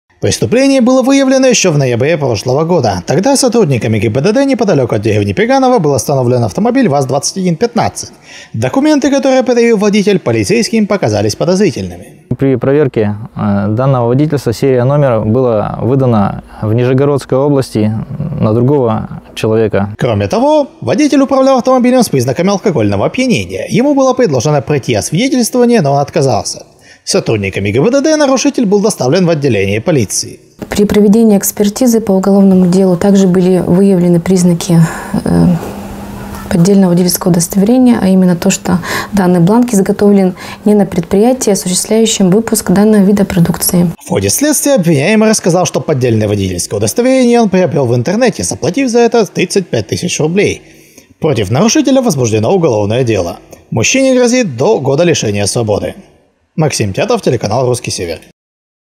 Купил права за 35 тысяч рублей
Ближайший год может провести в тюрьме устюжанин, пойманный с поддельным водительским удостоверением. Следственным отделом ОМВД России по Великоустюгскому району возбуждено уголовное дело.